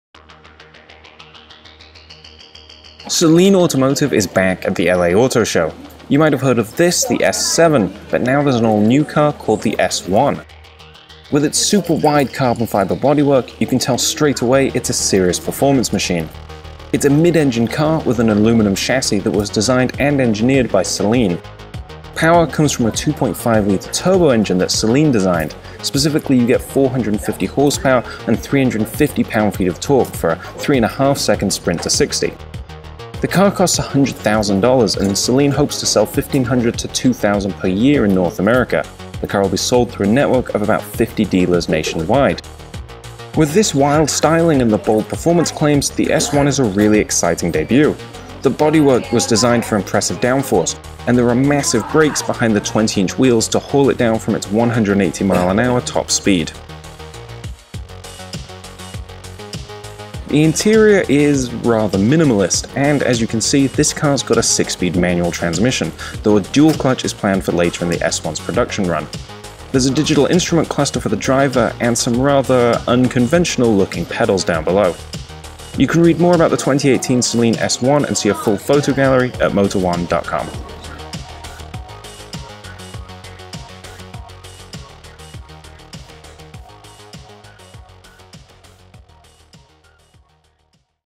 2018 Saleen S1: 2017 Los Angeles Auto Show
Saleen Automotive is introducing a new $100,000 supercar called the S1, which has carbon-fiber bodywork and a 450-horsepower turbo engine.

And for more 2017 Los Angeles Auto Show coverage stick